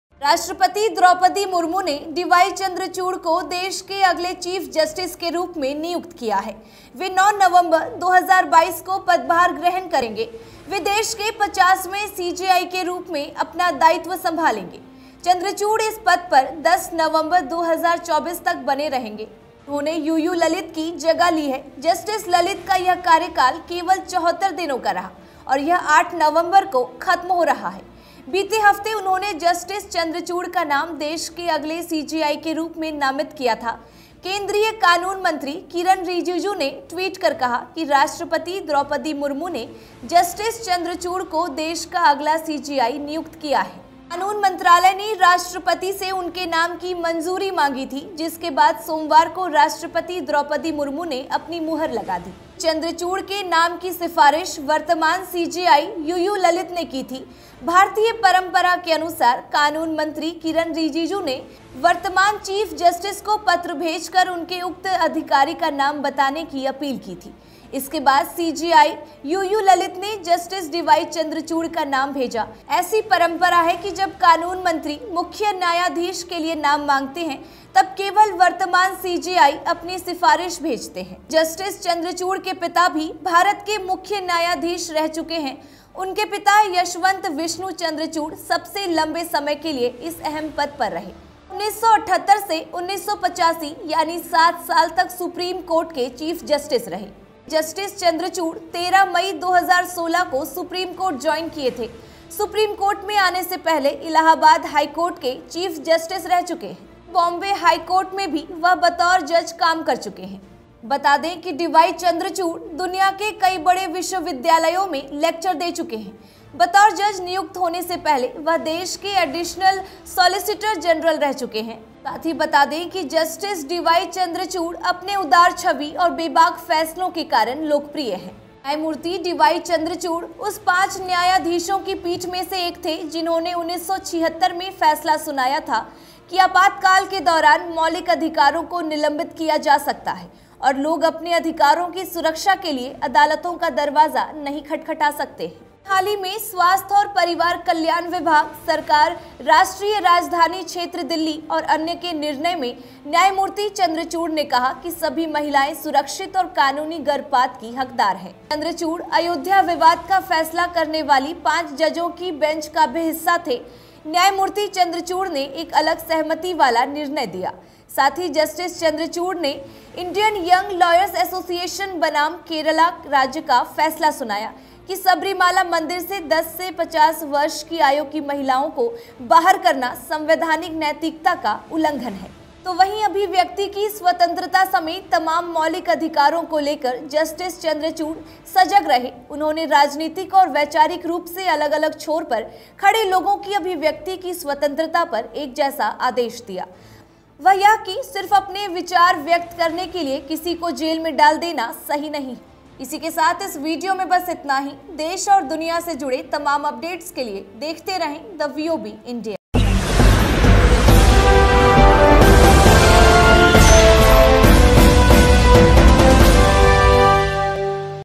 Next Chief Justice Of India : जस्टिस डीवाई चंद्रचूड़ के नाम पर राष्ट्रपति ने लगाई मुहर
राष्ट्रपति ने जस्टिस धनंजय यशवंत चंद्रचूड़ को देश का अगला चीफ जस्टिस नियुक्त किया है. 9 नवंबर से पद संभालने वाले जस्टिस चंद्रचूड़ देश के 50वें मुख्य न्यायाधीश होंगे